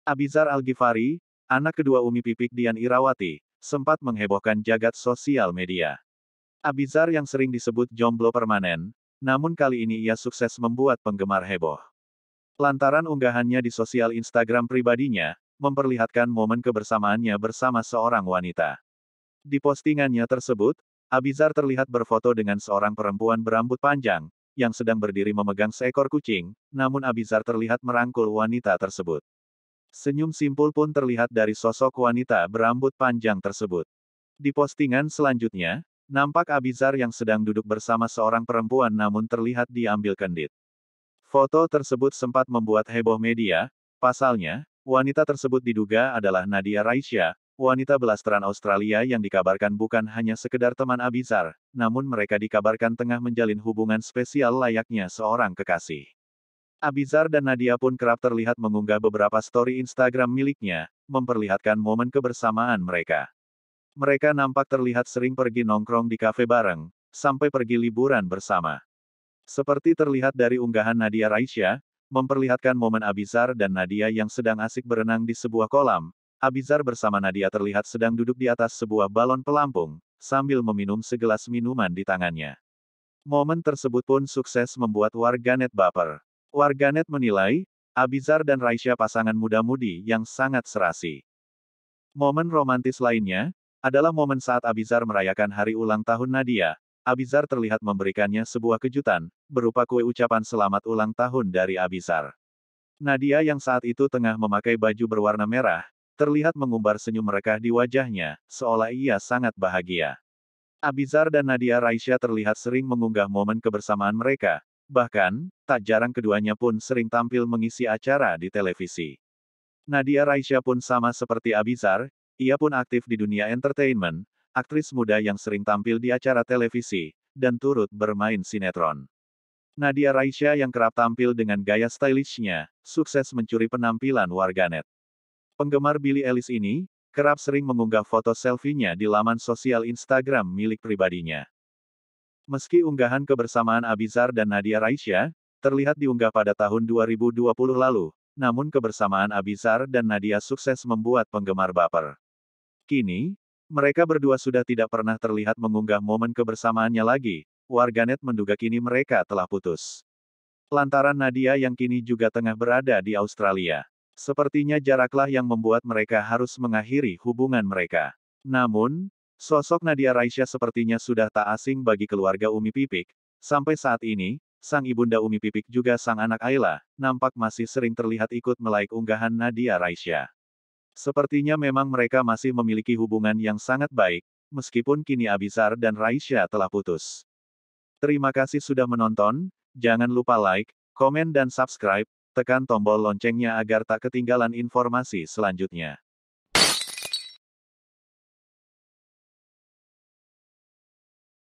ABIDZAR DAN NADIA RAISYA PACARAN, SUDAH GO PUBLIK!
Abidzar dan Nadia Raisya pasangan muda mudi yang serasi pada masanya yah.